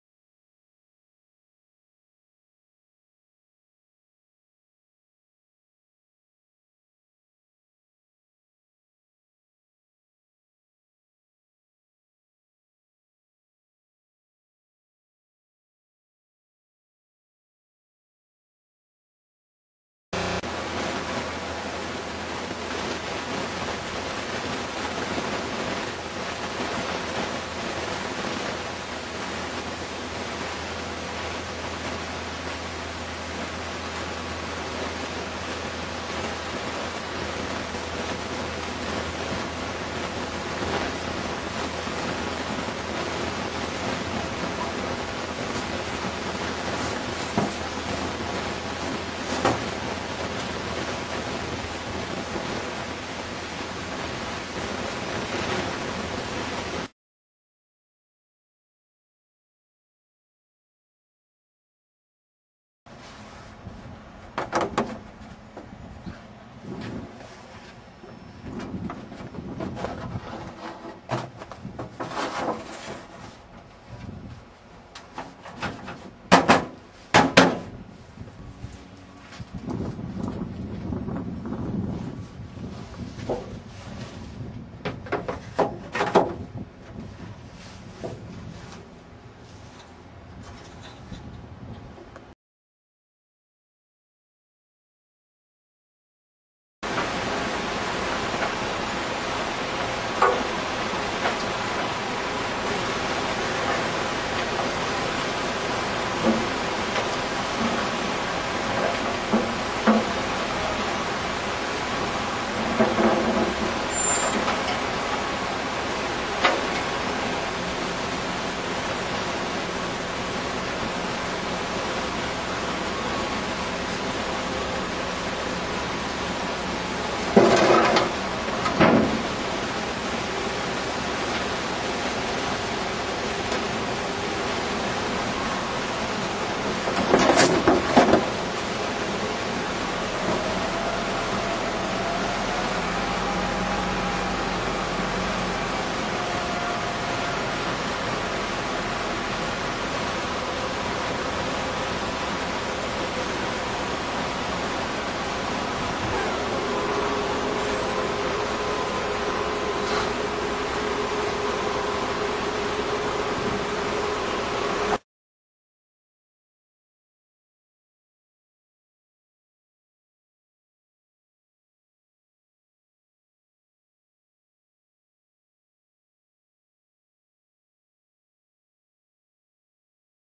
ULTRALINER
With the use of light weight thermoplastic liners and no additional labor, large plain concrete blocks (approx. 2.5' x 2.5' x 5') are easily transformed into blocks with attractive architectural patterns.